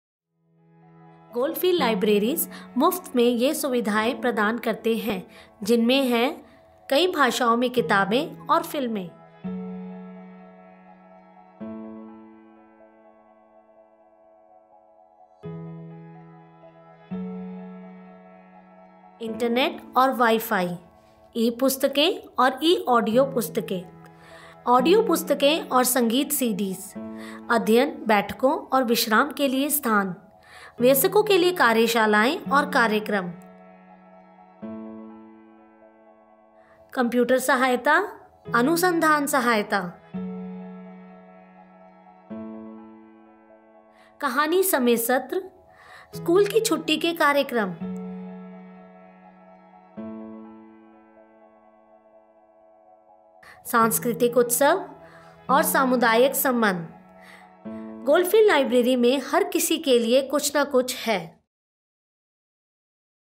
Goldfields Libraries - welcoming our multicultural communities (Hindi)
Welcoming and inclusive spaces are a key priority for Goldfields Libraries. This short video aims at introducing multicultural communities to the services and supports available to them through their local library.

Goldfields Libraries offer FREE:
• books and movies in many languages
• internet & Wi-Fi
• e-books and e-audiobooks
• audiobooks and music CDs
• spaces for study, meetings…and relaxation
• workshops and events for adults
• storytime sessions
• school holiday activities
• computer help
• research help
• cultural celebrations
• community connection
• home library deliveries

There’s something for everyone at Goldfields Libraries. Be part of our story!